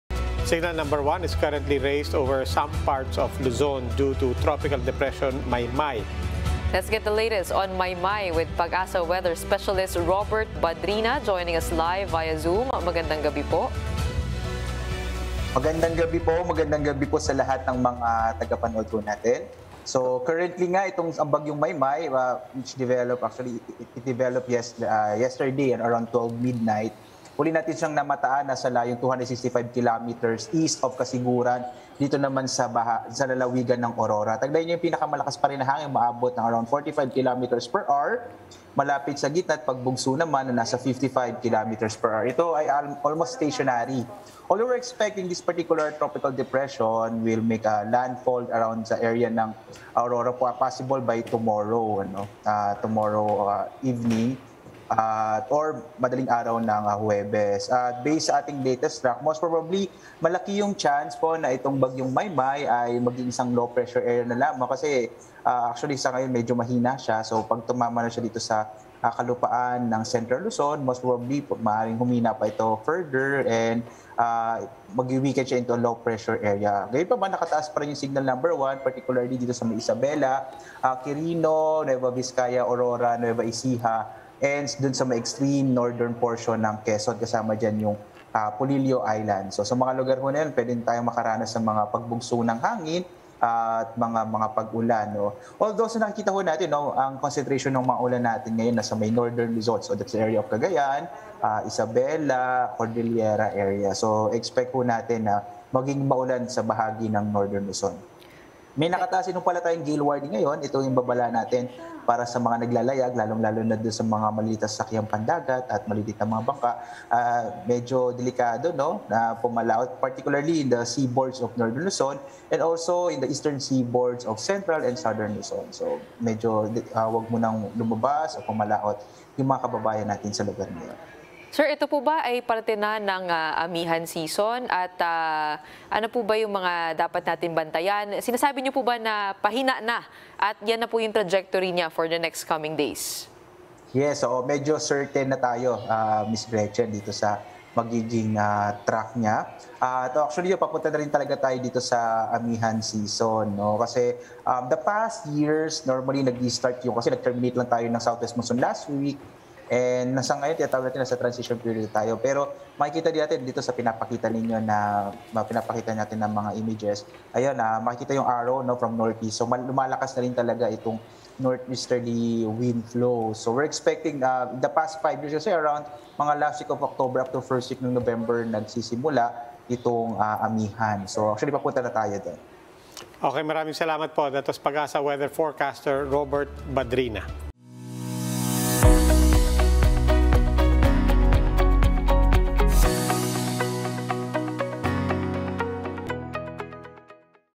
Tropical Depression MaymayPH almost stationary
The cyclone is expected to make landfall over Aurora by Wednesday evening or Thursday morning, it will weaken as it crosses landmass, according to PAGASA weather specialist Robert Badrina.